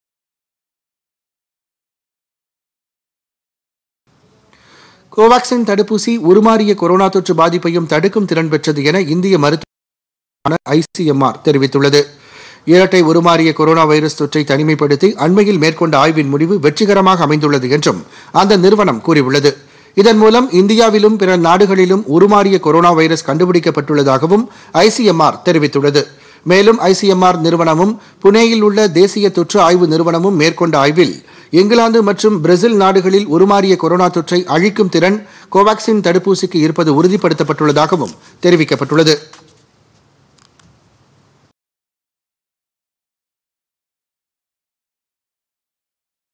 உருமாறிய தொற்றையும் கோவேக்சின் அழிக்கும்PodhigaiTamilNews
இரட்டை உருமாறிய கொரோனா தொற்றையும் தடுக்கும் திறன் பெற்றது கோவேக்சின் தடுப்பூசி - இந்திய மருத்துவ ஆராய்ச்சிக் குழுமம் அறிவிப்பு.